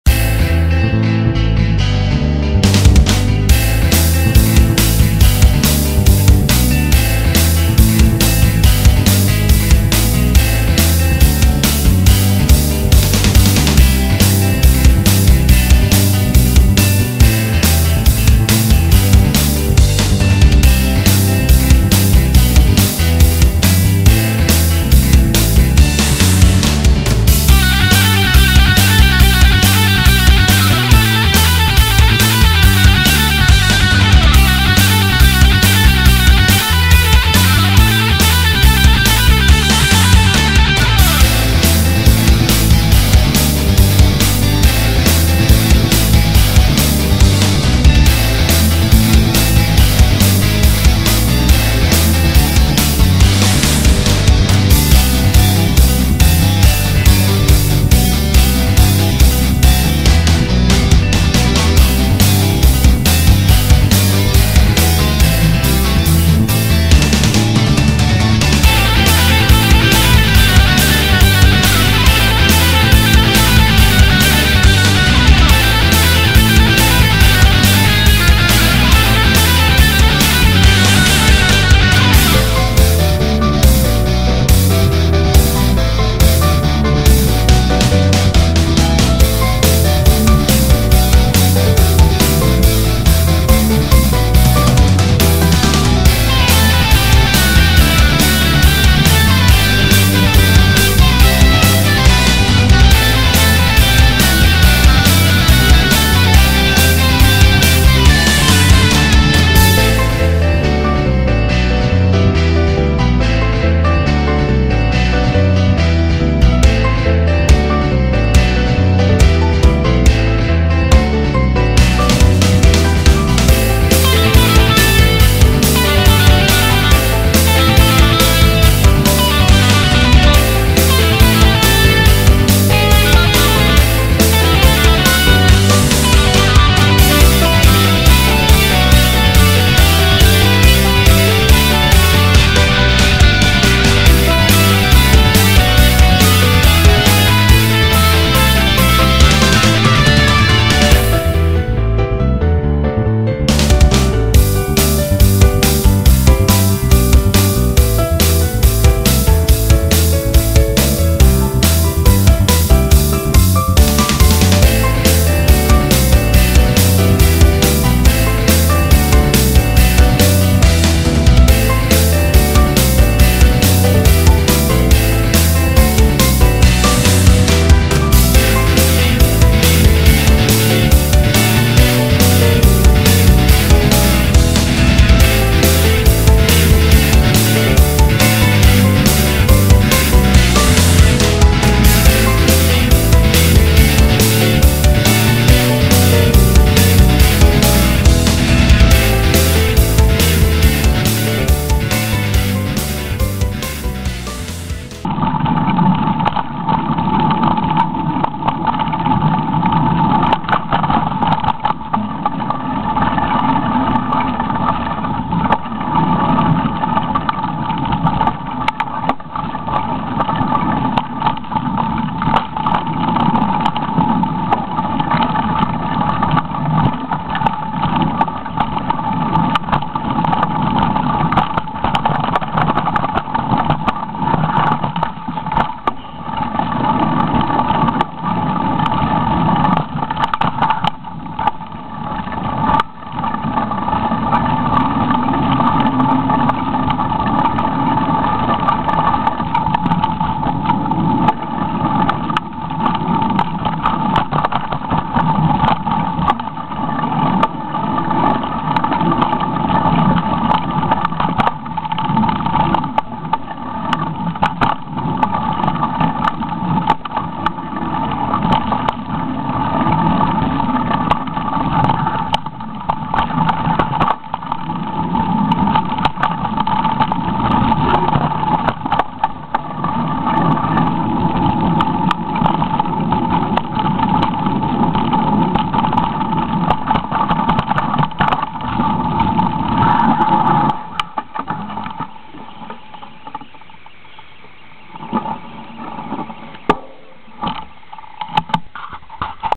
The beast single track at high lodge
The beast on the black run at high lodge . Filmed at 30 frames per second . So seems slow but anything but.